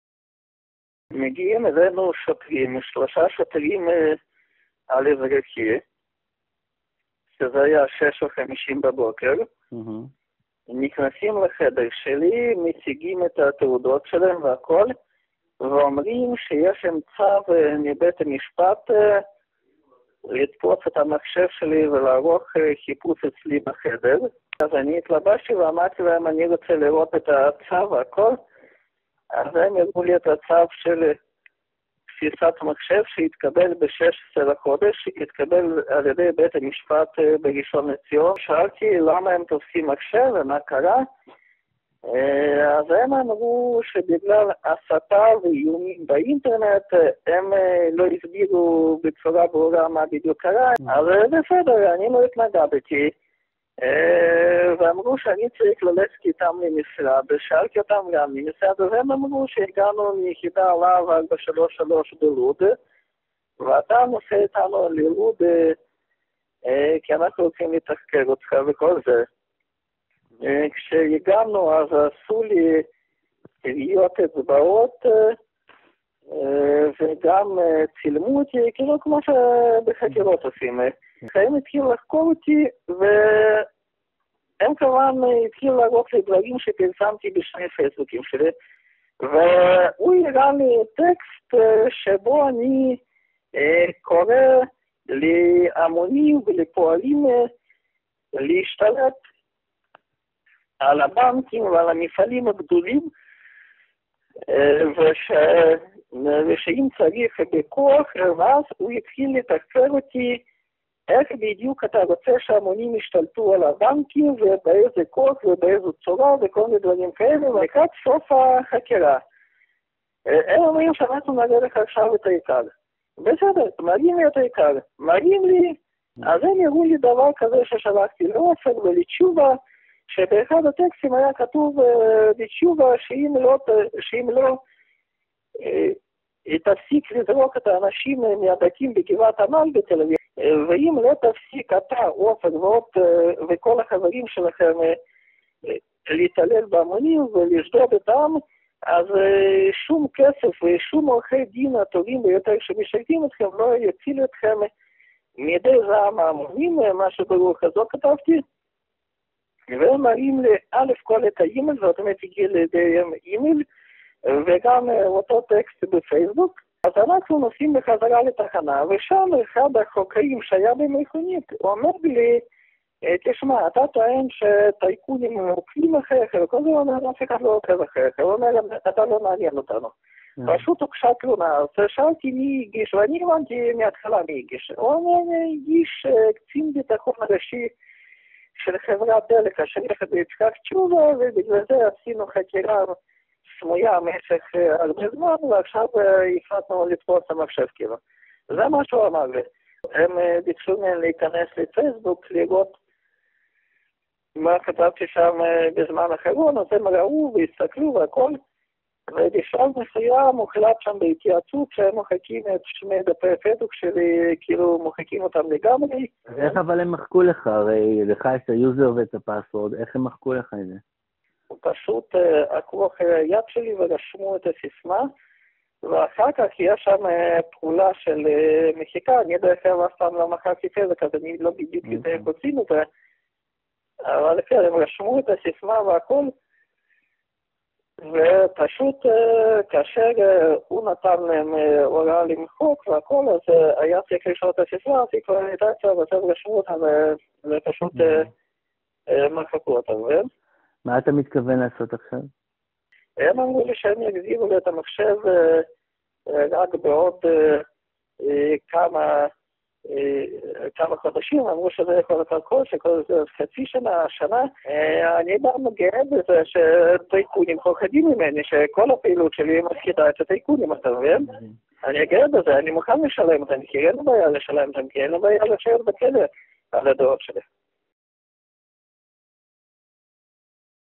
שמואל ירושלמי על מעצרו ומחיקת דפי הפייסבוק (עם כתוביות)
שמואל ירושלמי, פעיל חברתי ומשורר מבאר שבע כתב בפייסבוק נגד יצחק תשובה. תלונה שהוגשה נגדו במשטרה הובילה למעצרו ולחקירתו ביחידת להב 433. מחשבו הוחרם והדפים שלו בפייסבוק נמחקו בידי חוקרי המשטרה. ראיון אישי (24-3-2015).